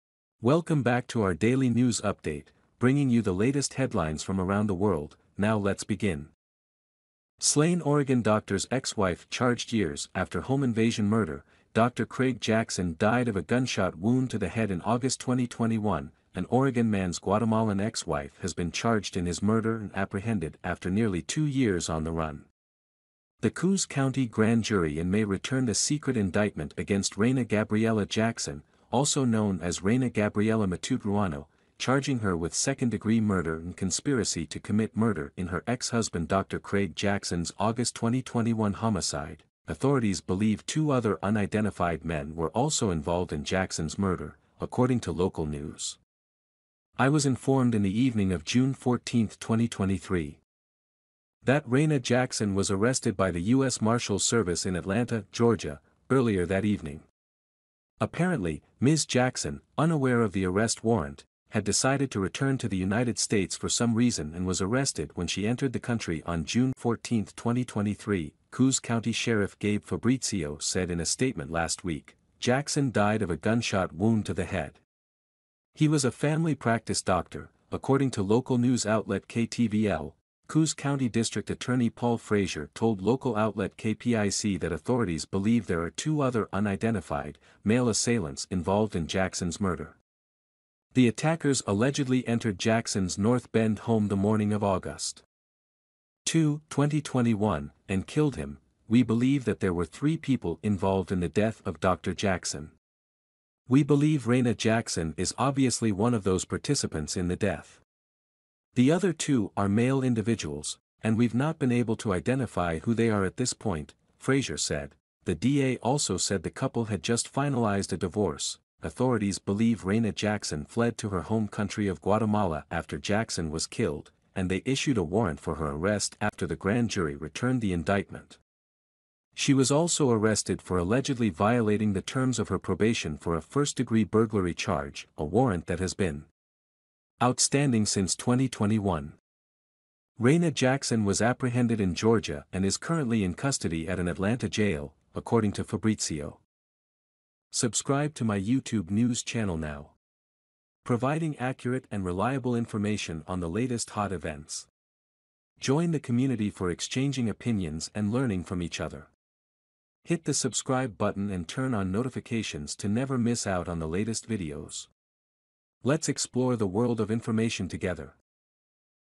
Slain Oregon doctor's ex wife charged years after home invasion murder
Dr. Craig Jackson died of a gunshot wound to the head in August 2021